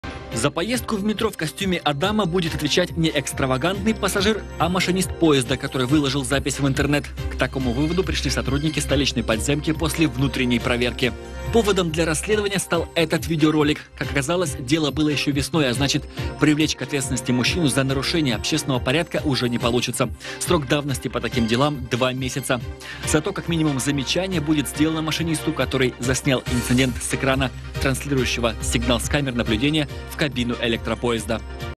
Нудист в метро.
Голый человек прокатился в Московском метрополитене.